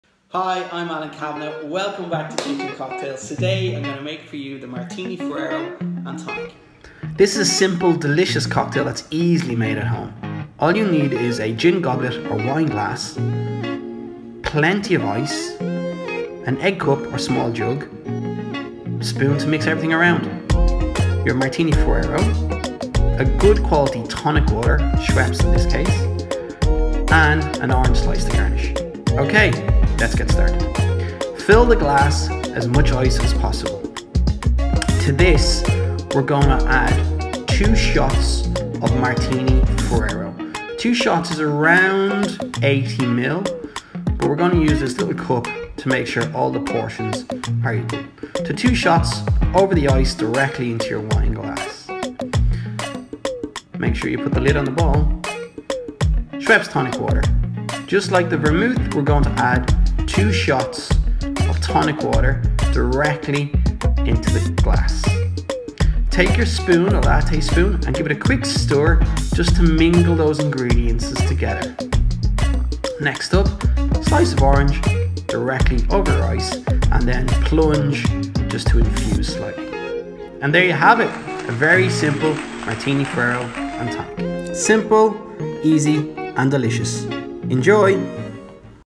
Martini Fiero & Tonic by Kitchen Cocktails
A secret blend of white wines and botanicals such as zesty mandarin and sweet Spanish oranges. When mixed with tonic, this creates the perfect modern, simple cocktail with a low ABV rating of 3.75%.

Ingredients:
80ml Martini Fiero
80ml tonic water

Garnish:
Orange slice

Method:
Add chosen vermouth into a
balloon glass over ice and top
with tonic water. Garnish with an
orange slice.